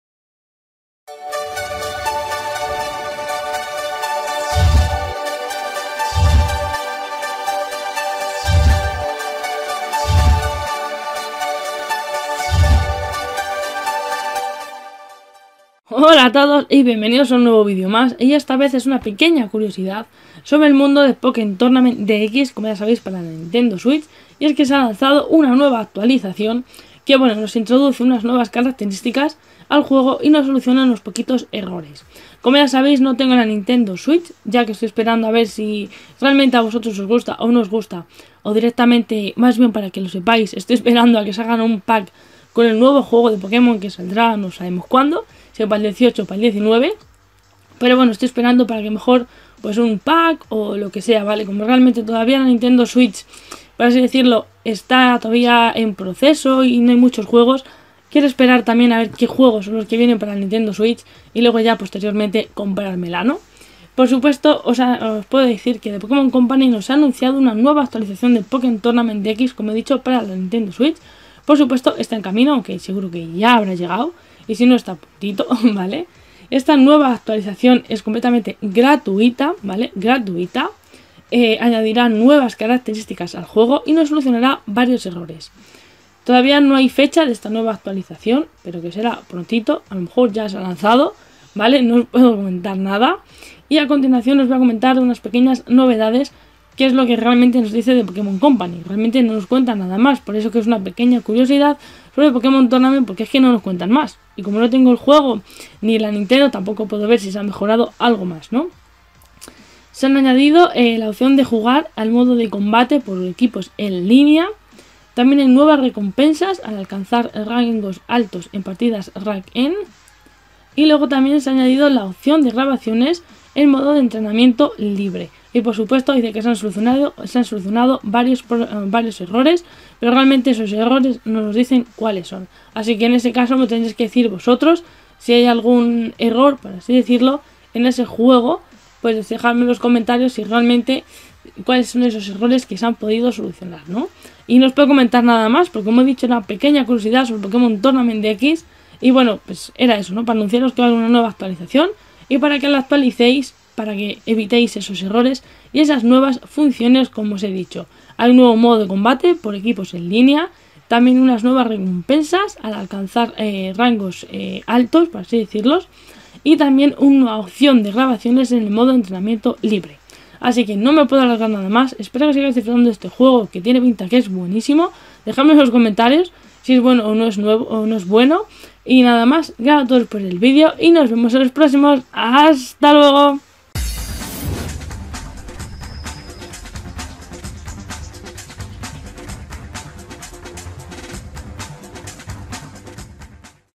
Anunciada actualización importante de Pokkén Tournament DX
Os cuento un poquito sobre la actualizacion del nuevo juego de Pokémon para la Nintendo Switch.